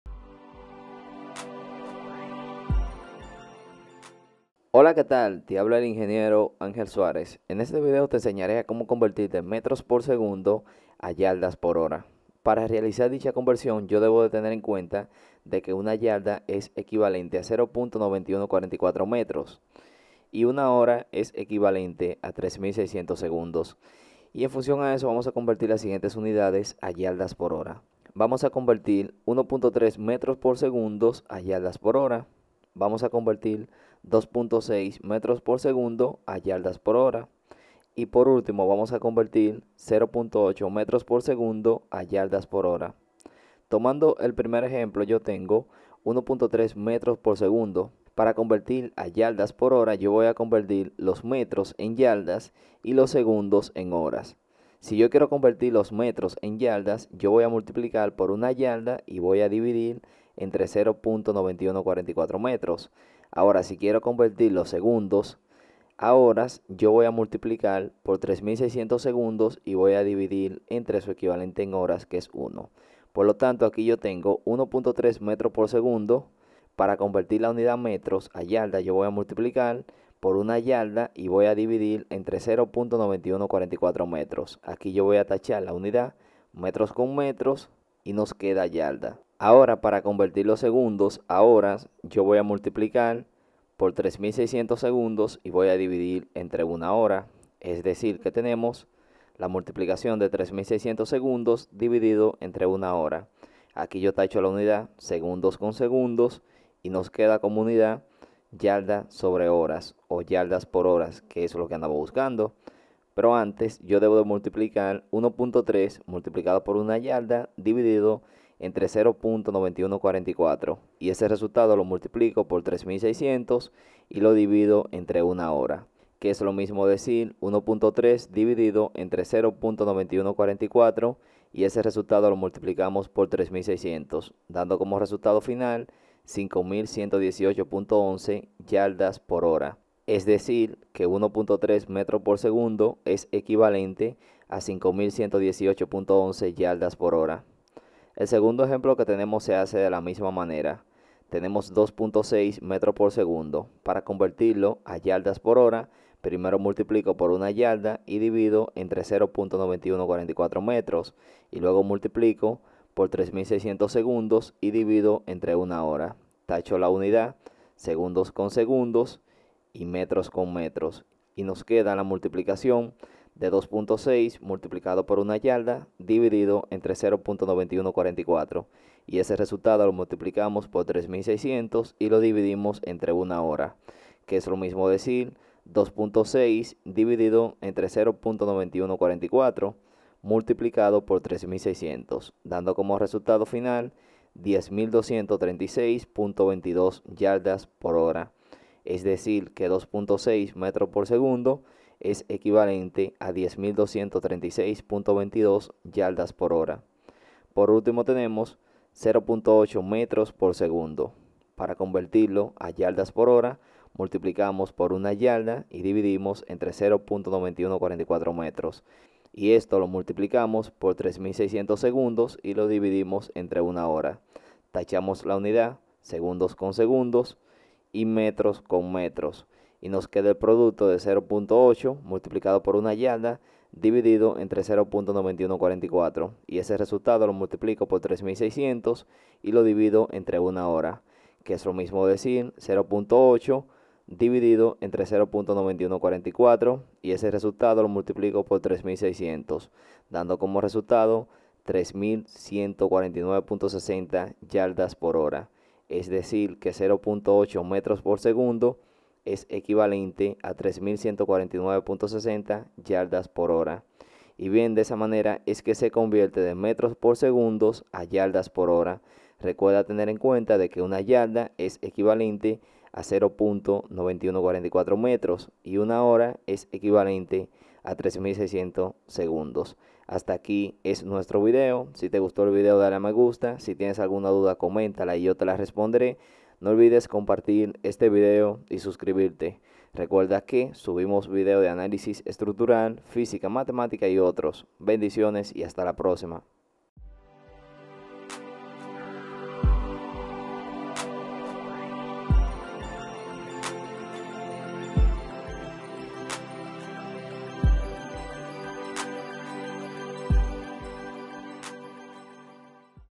Metros por segundo a Yardas por hora (m/s a yd/h)
Metros por segundo a Yardas por hora

En este video el ingeniero Angel Suárez explica tres ejemplos de como realizar la conversión de Metros por segundo a Yardas por hora, en función a los factores de conversión de las mismas unidades.

¡Sígueme!
iEnciclotareas- Canal de Ejercicios Resueltos de Ingeniería Civil